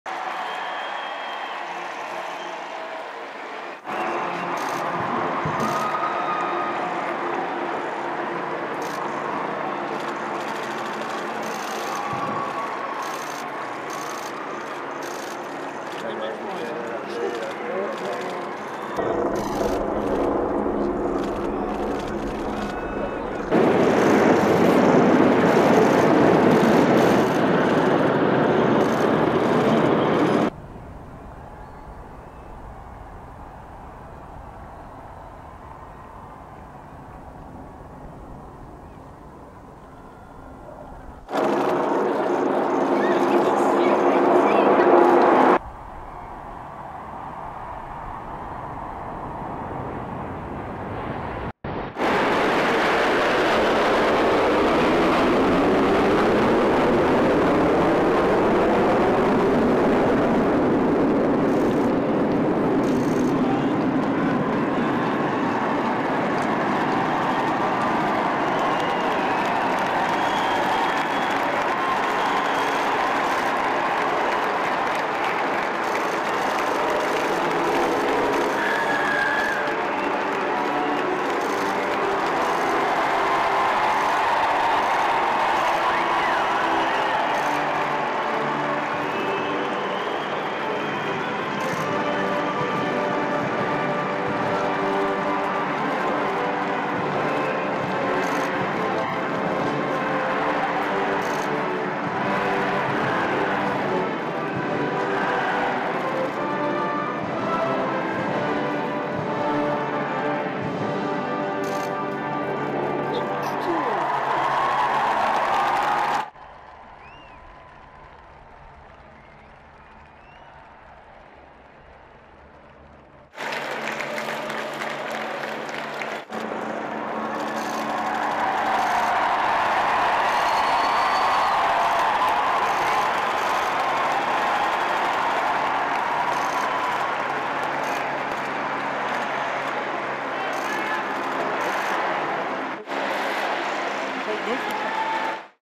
Queen's Platinum Jubilee: Royal Family make classic balcony appearance after military parade
Once the Trooping the Colour parade ended and the military procession returned to Buckingham Palace in London, England, the Royal Family made a balcony appearance as they watched the flyover, as they do every year.

Only working royals have been invited to stand on the balcony this year, leaving out Prince Harry and Meghan Markle. The Queen’s embattled son Prince Andrew has also been left off the invite list after being stripped of his royal title and patronages earlier this year.

Queen Elizabeth II's annual birthday parade kicked off four days of Platinum Jubilee festivities on June 2.

The Queen is the first British monarch to celebrate 70 years of service and will be honoured for the work she’s done for the people of the United Kingdom, the Commonwealth and the Realms.